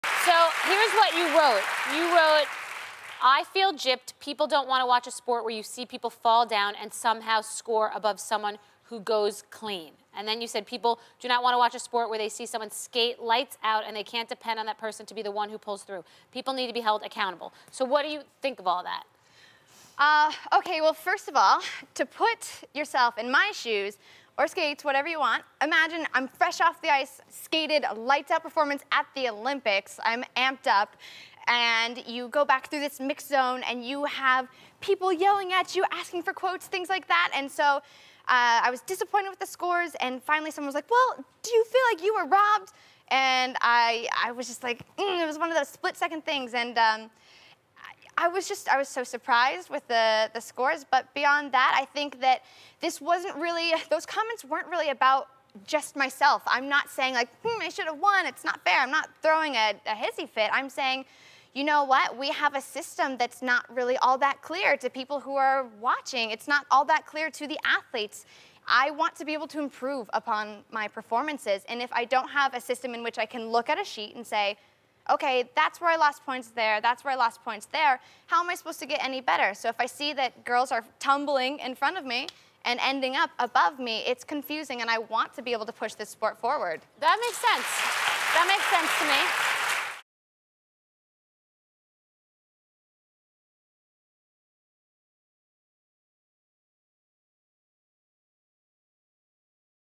Ashley Wagner Talks About Her Controversial Tweets
The Olympic medalist says some of her tweets were a little amped-up.